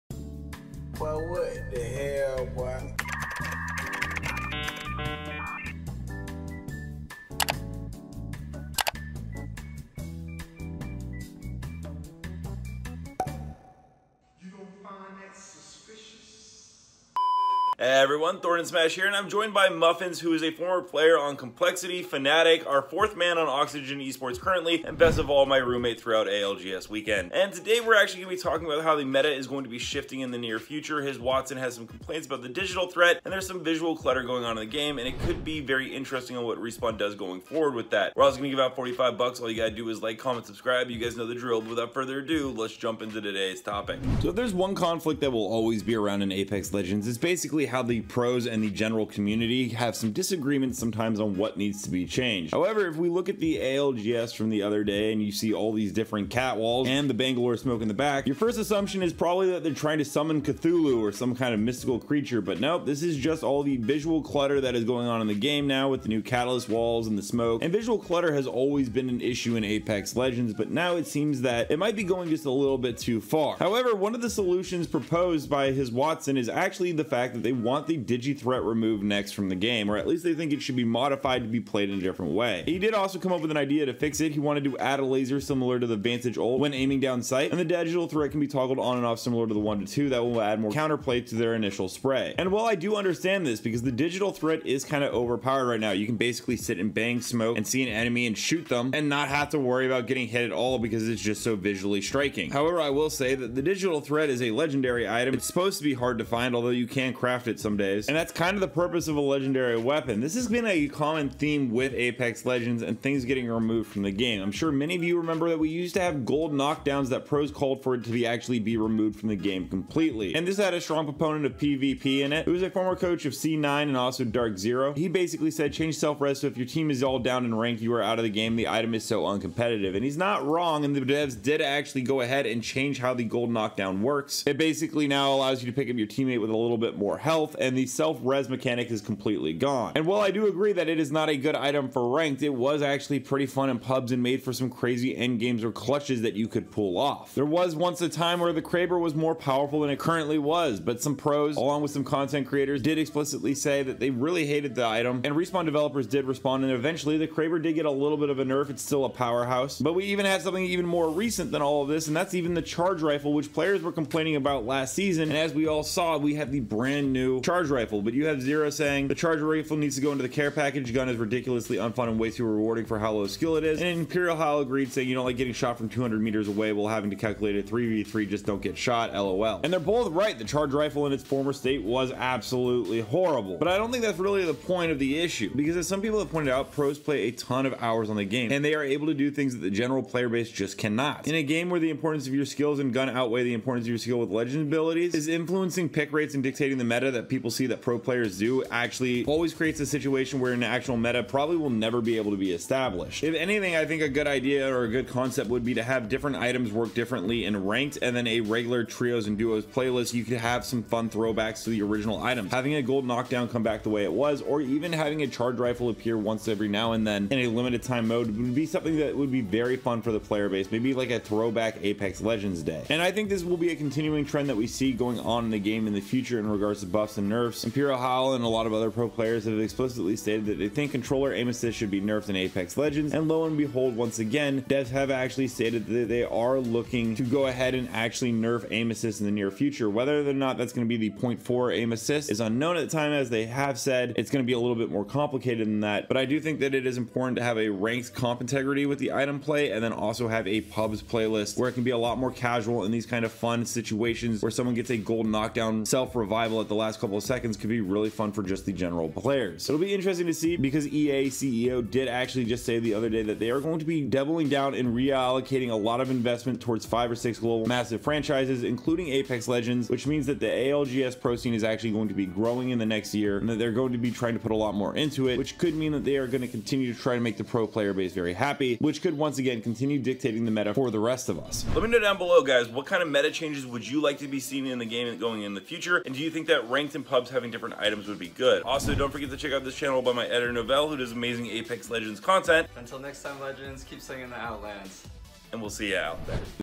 Apex Legends New Meta Is Getting A Huge Nerf
Apex Legends New Meta Is Getting A Huge Nerf...Former Apex Legends professional player, Jacob "HisWattson" McMillin took to X to voice his frustration with the overpowered attachment, urging Respawn Entertainment to nerf Digital Threat scopes. HisWattson posted, "The Digital Threat is low counterplay and you don't know [opponents] have one on until they've already shot half their mag at you." He continued, "Very frustrating to get randomly beamed through a smoke especially early game." The Apex Legends community shares his opinion in the replies, voicing their discontent with the attachment.